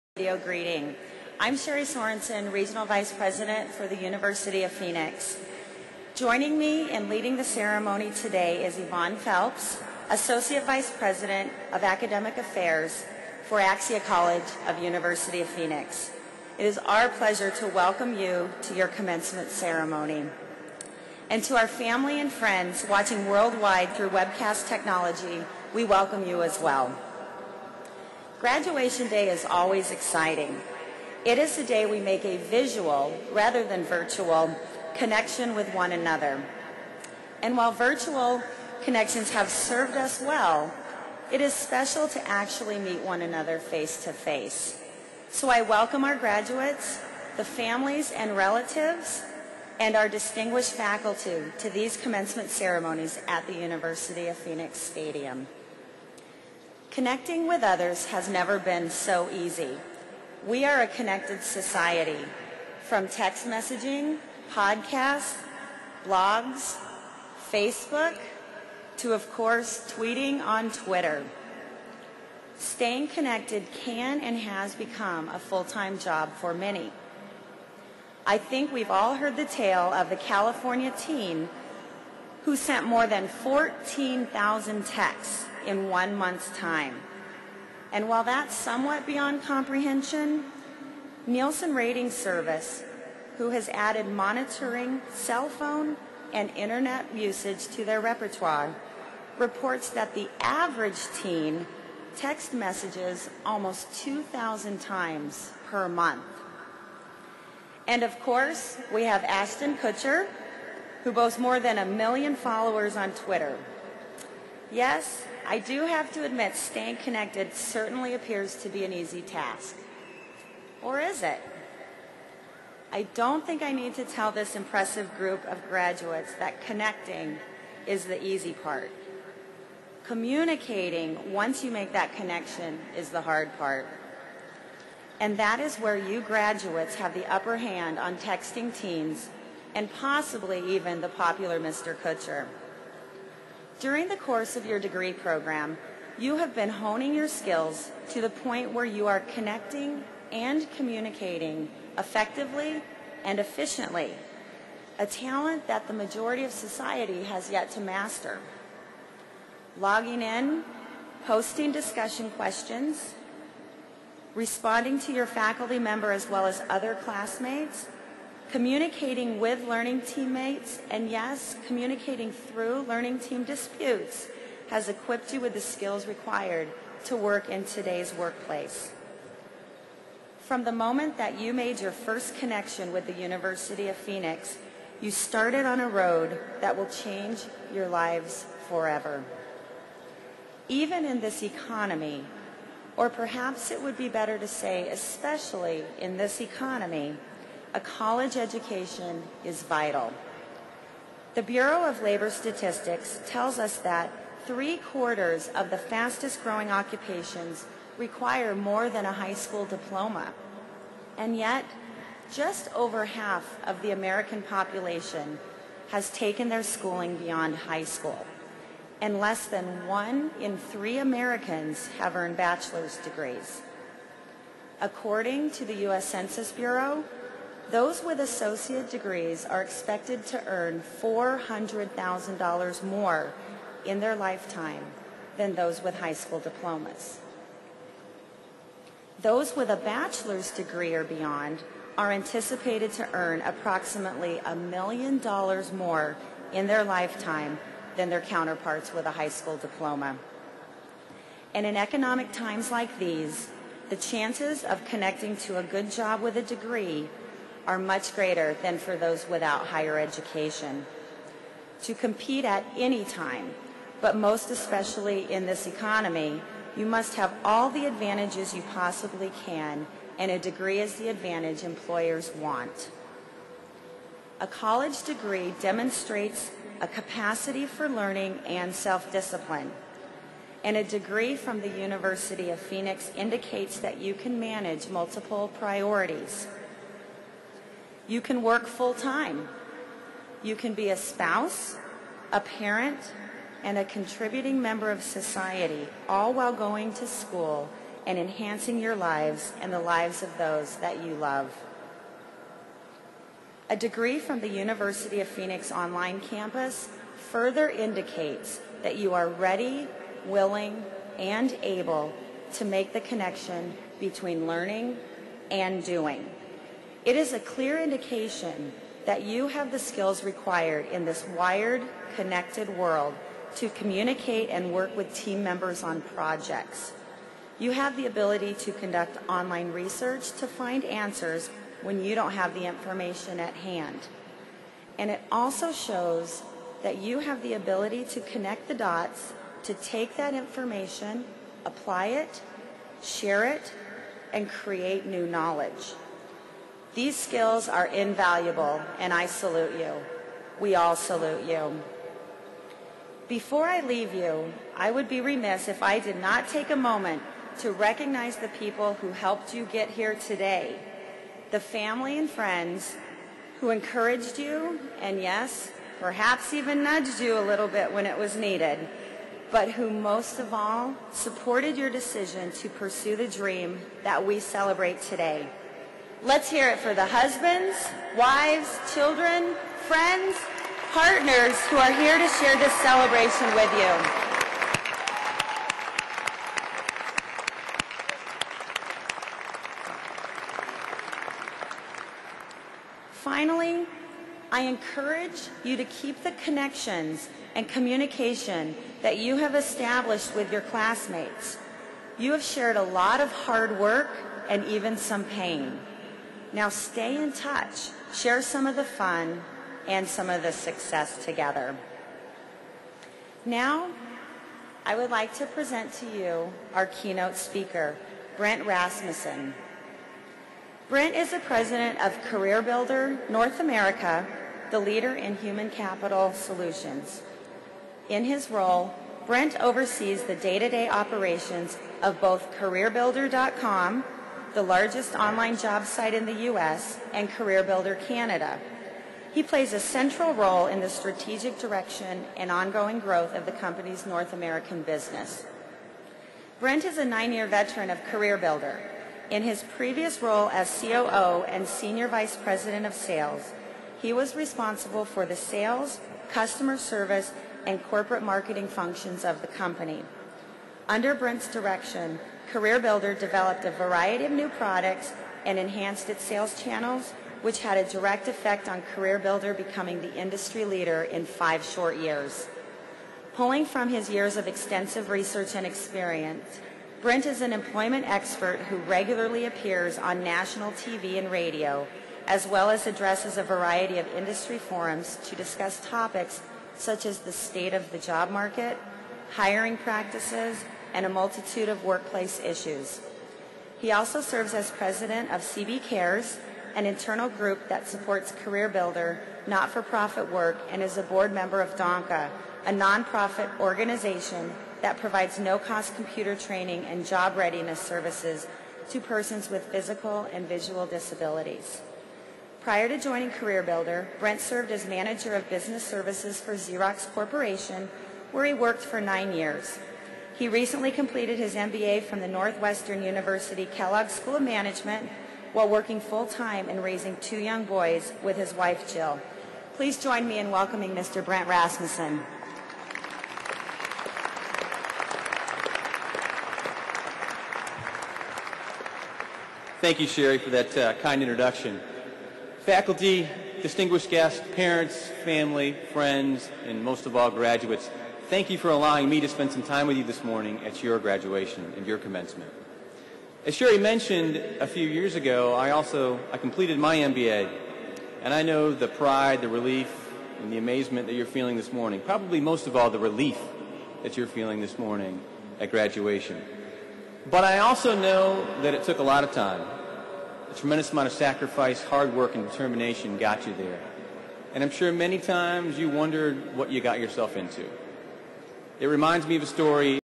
University of Phoenix - Celebrate the Class of 2009 Pt3
University of Phoenix Stadium in Phoenix.
Axia College of University of Phoenix Friday, July 17, 2009 at 10:30 a.m. AZ Time